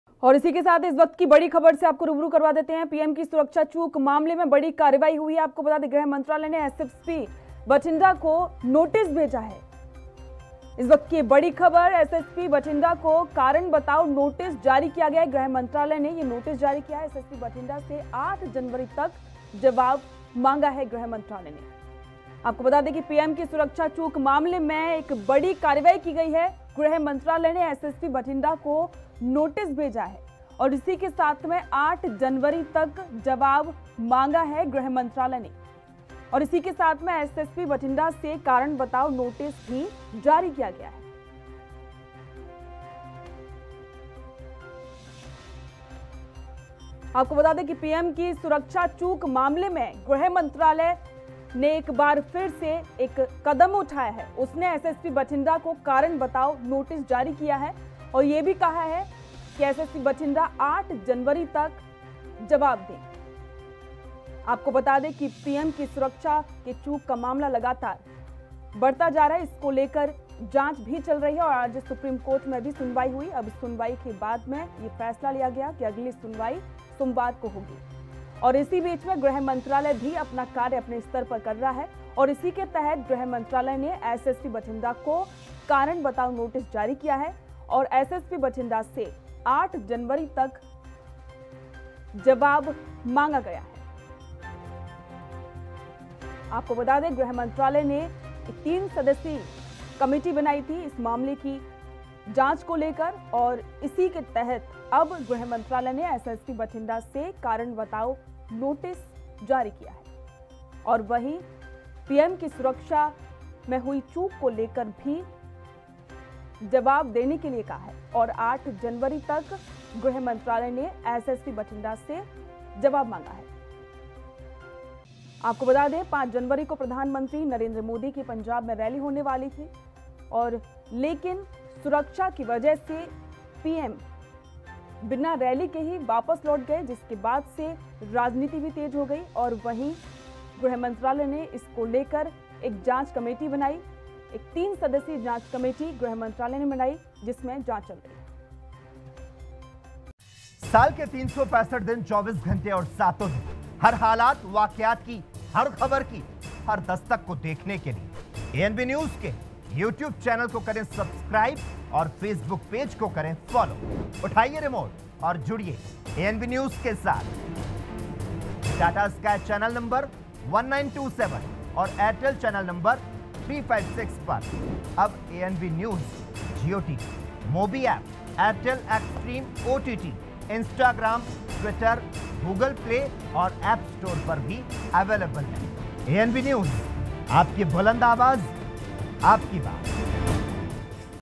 पीएम की सुरक्षा चूक मामले में बड़ी कार्रवाई गृह मंत्रालय ने SSP भटिंडा को भेजा नोटिस | ANB NEWS
पीएम की सुरक्षा चूक मामले में बड़ी कार्रवाई गृह मंत्रालय ने SSP भटिंडा को भेजा नोटिस SSP भटिंडा को कारण बताओ नोटिस जारी SSP भटिंडा से 8 जनवरी तक जवाब मांगा